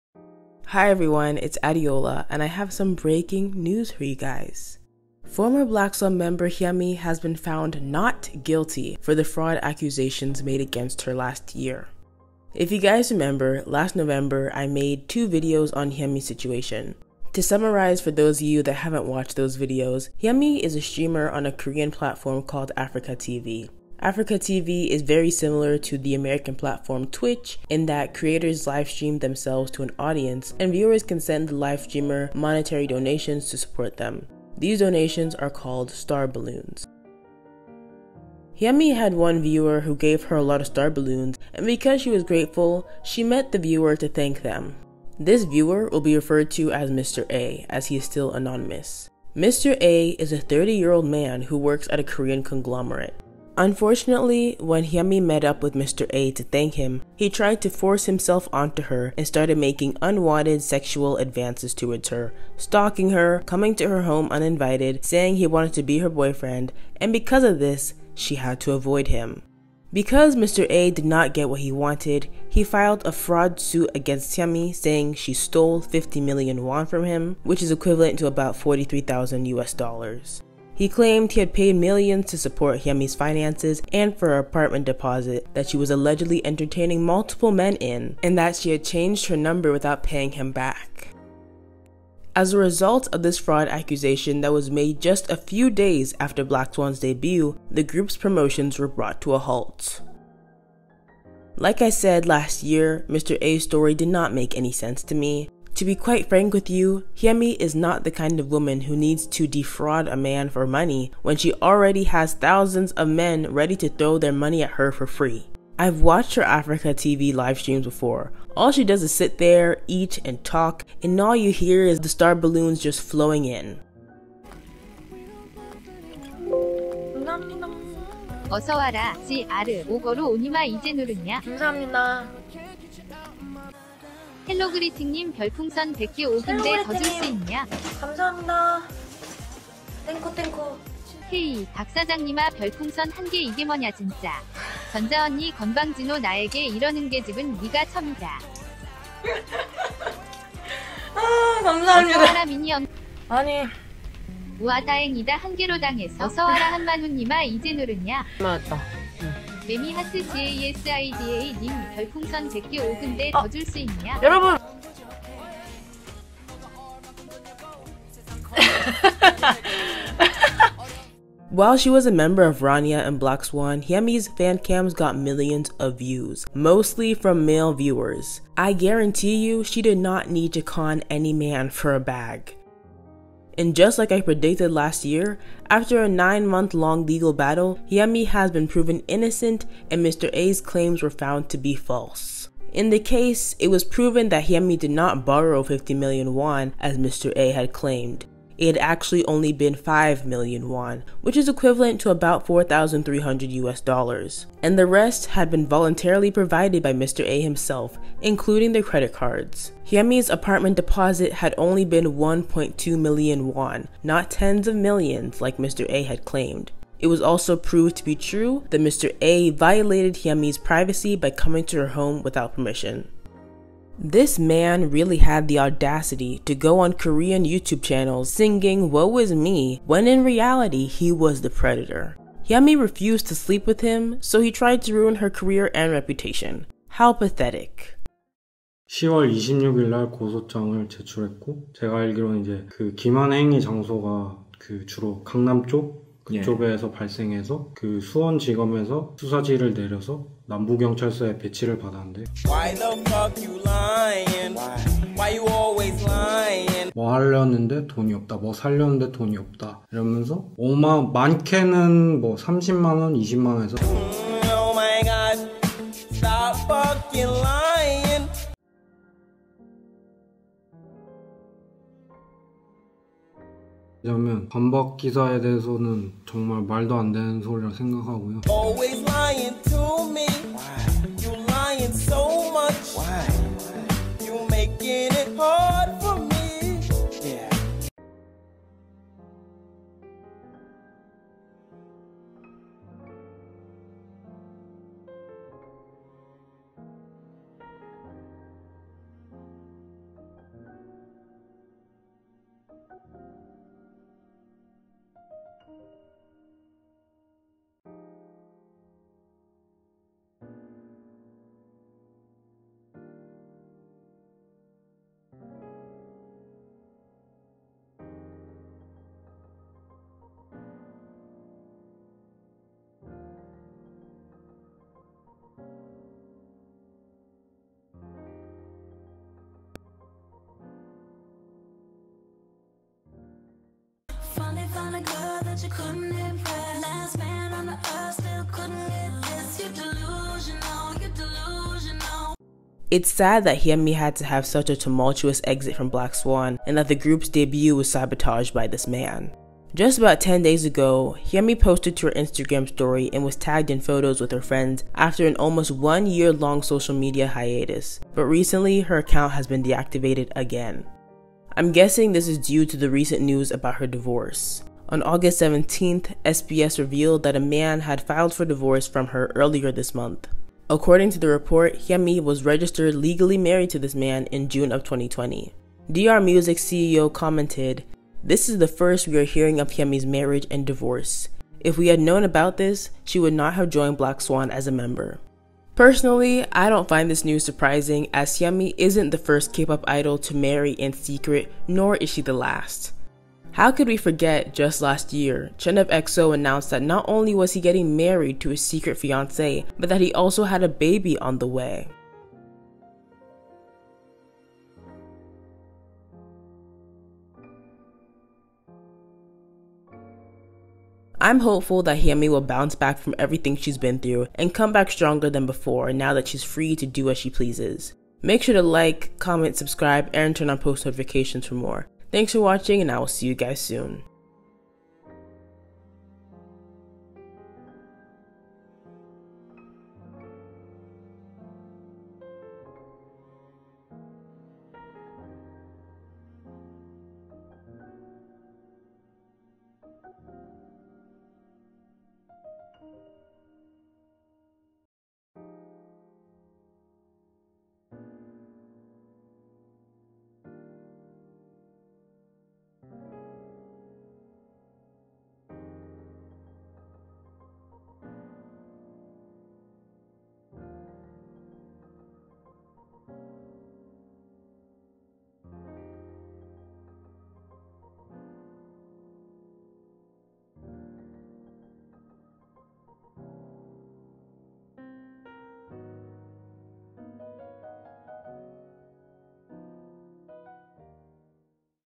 Ex-BLACKSWAN member Hyeme found NOT GUILTY of fraud + her alleged STALKER 🔍 [혜미 블랙스완]
Note: the audio for this video was recorded August 19th (for reference when I say things like “about 10 days ago”).

단독] 블랙스완 출신 혜미, 5000만 원 사기 논란 '혐의 없음' 결론